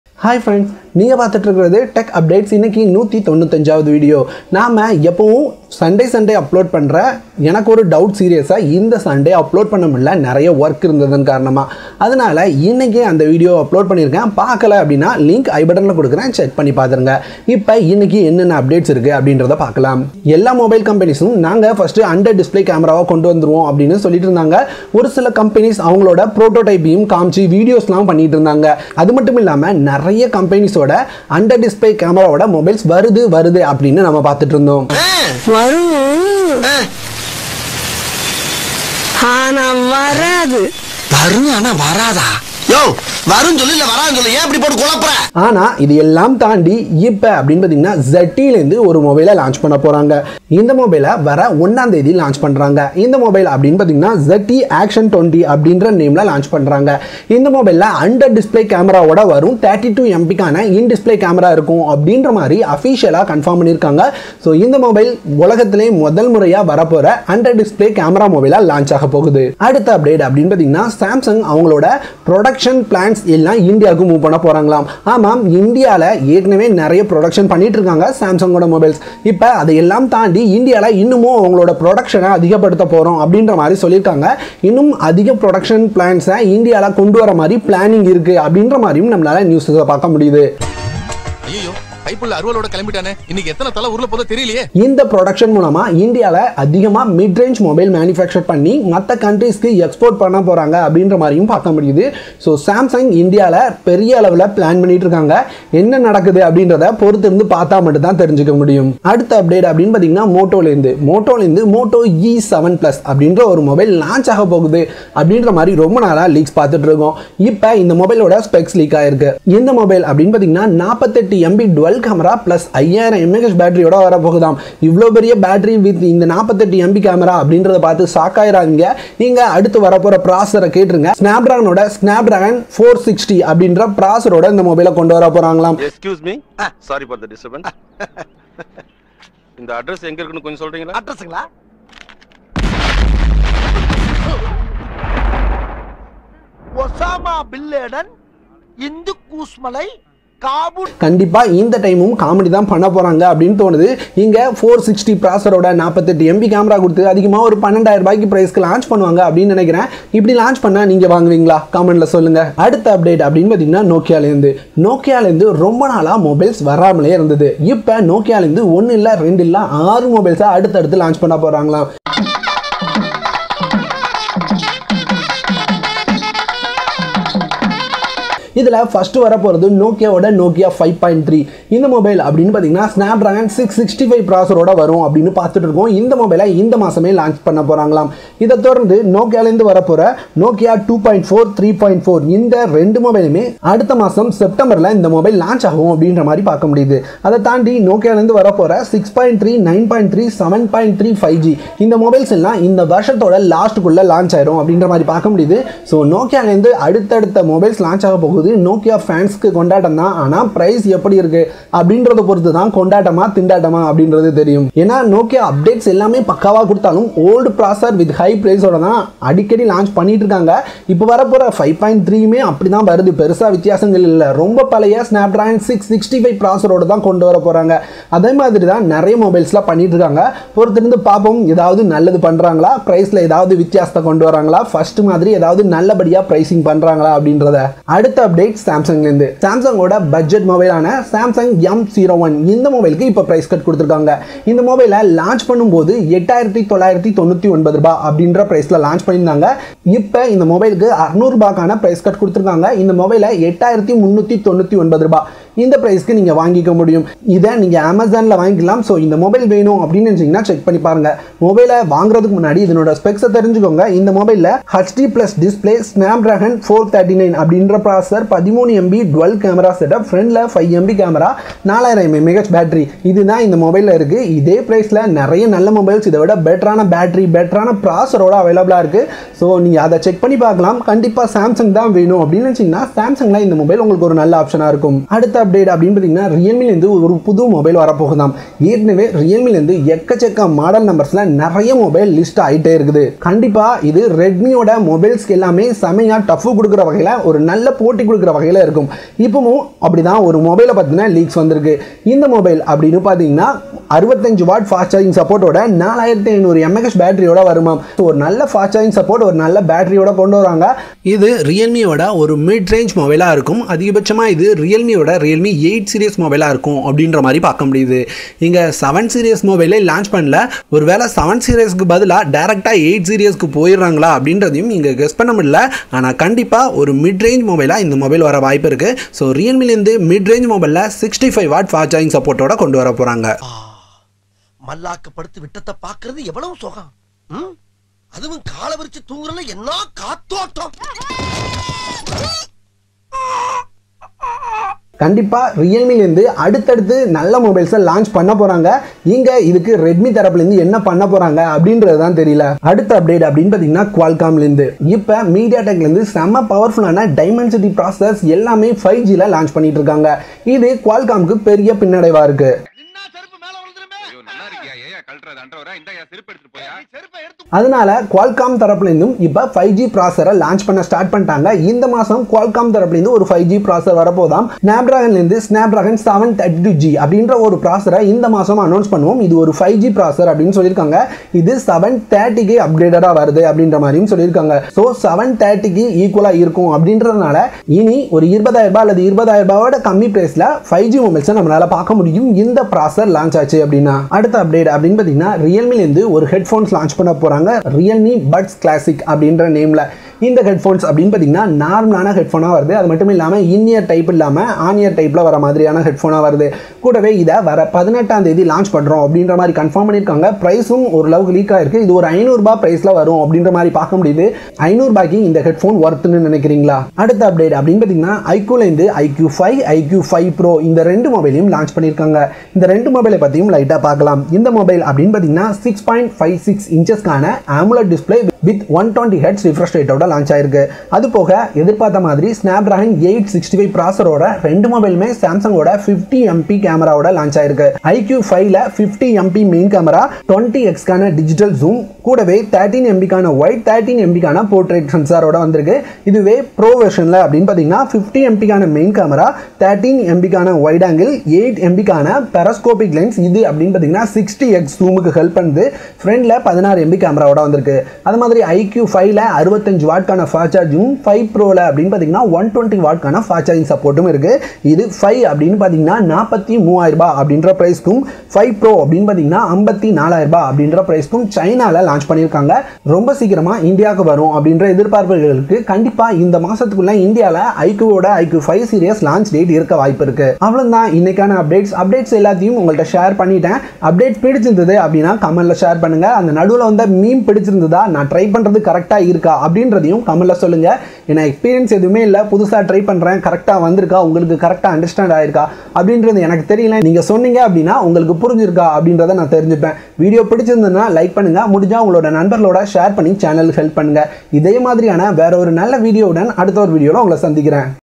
Realme 8 Specs,Realme Classic Launch,iQoo 5&5 Pro Launched,Qualcomm 732G Launch,Nokia 6 Mobiles #195
ஏதேனும் சந்தேகம் இருந்தால் என்னை Facebook Pageயில் அனுகவும்

பட்ஜட் கட்ஜட் தமிழ் - Budget Gadget Tamil